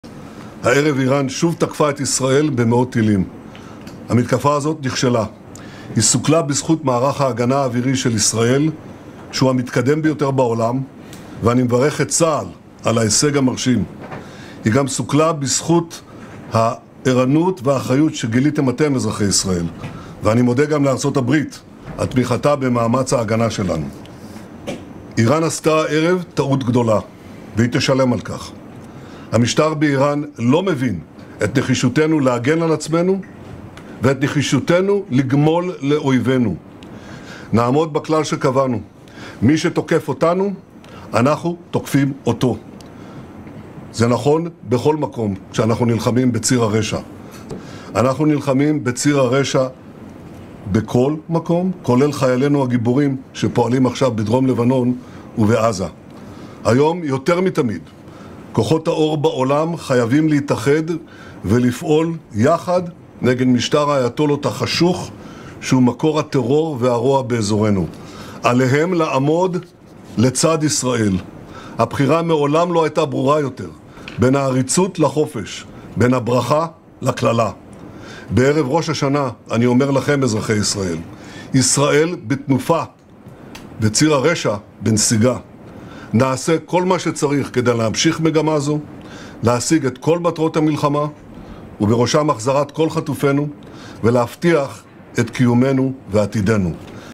“O Irã cometeu um grande erro e irá pagar por isso”, diz Netanyahu após ataque de Teerã
Em pronunciamento, primeiro-ministro de Israel afirmou que ataque iraniano foi frustrado pelo sistema de defesa israelense e agradeceu o apoio dos EUA.

(Via: @samuelpancher)

🎥 Reprodução